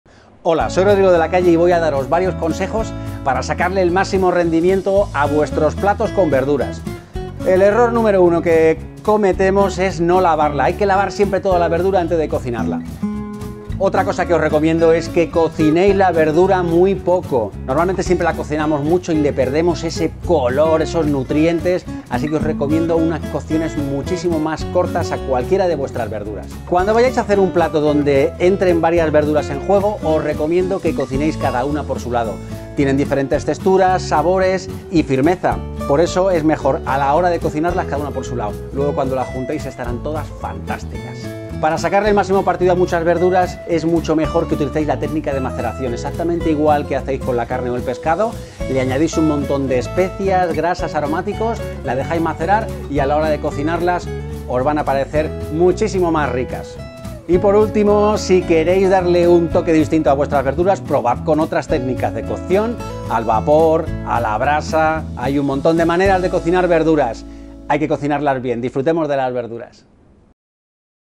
5 consejos para cocinar verduras correctamente - Rodrigo de la Calle.
👨‍🍳¿Sabías que cocinar las verduras correctamente es clave para disfrutar de todo su sabor y obtener todos sus beneficios?   💯

🆕 Hoy, el Chef Verde y Estrella Michelín, Rodrigo de la Calle, nos aporta una serie de recomendaciones para preparar verduras de la forma correcta. ✅, ¡No te los puedes perder! ¡Toma nota! 📝

1. Lavar  siempre nuestras verduras antes de cocinarlas.
2. Cocinar las verduras menos tiempo.
3. Cocinar las verduras por separado.
4. Macerar nuestras verduras.
5. Probar nuevas técnicas de cocción

💚 ¡Disfrutemos de las verduras! 🥬